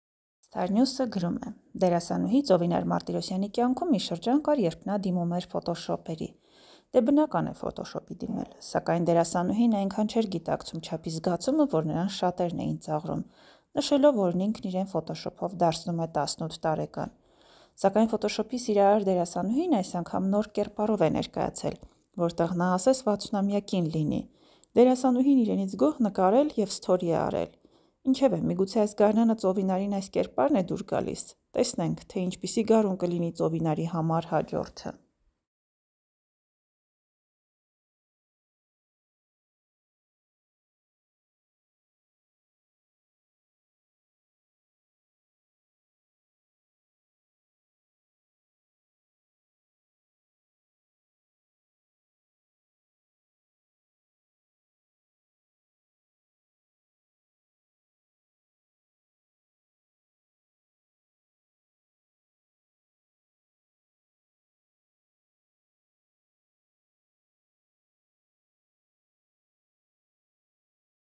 Ժամանակին ֆոտոշոփի գերի դարձած Ծովինար Մարտիրոսյանը նմանվել է 60-ամյա կնոջ
Աղբյուրը` starnews.am